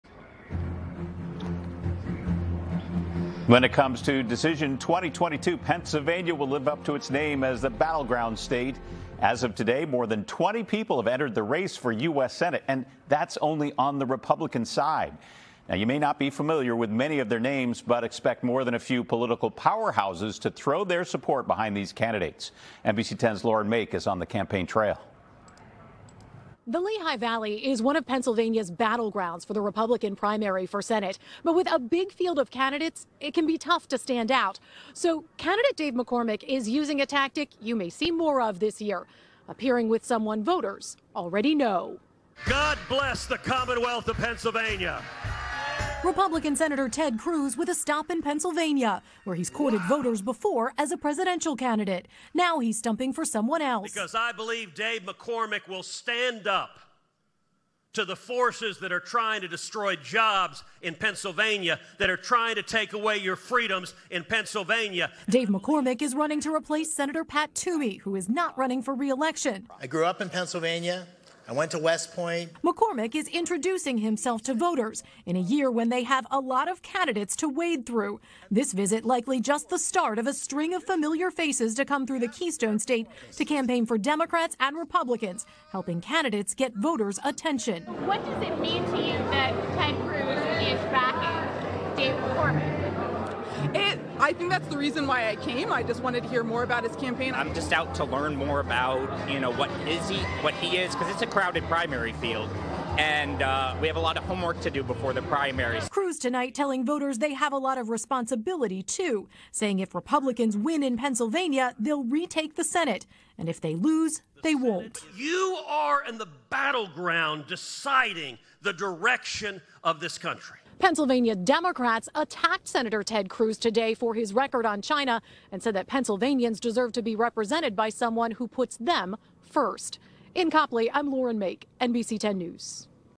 Ted Cruz Speaks at Dave McCormick Rally in Lehigh County
More than 20 GOP candidates have entered the Pennsylvania senate race. NBC10’s Lauren Mayk was in the Lehigh Valley as Senator Ted Cruz endorsed candidate Dave McCormick.